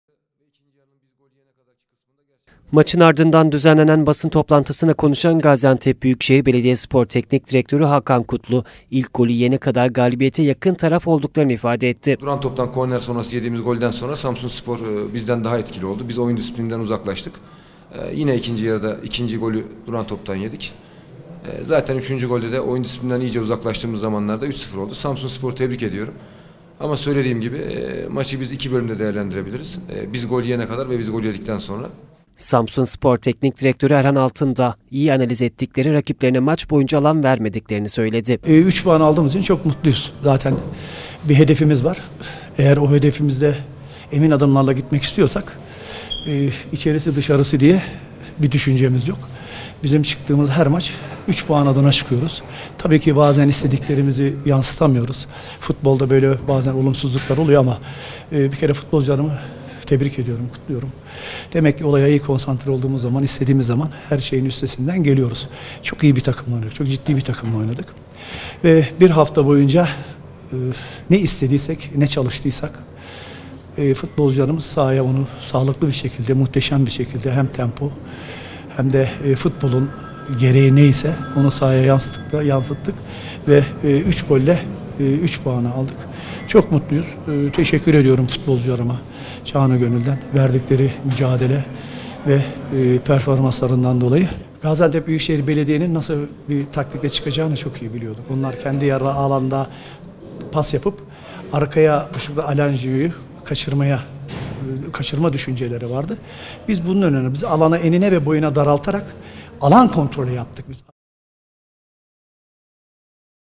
ANTEP'TE GÜLEN TARAF SAMSUNSPOR
PTT 1. Lig'in 28. haftasında Samsunspor, deplasmanda Gaziantep Büyükşehir Belediyespor'u 3-0 mağlup etti.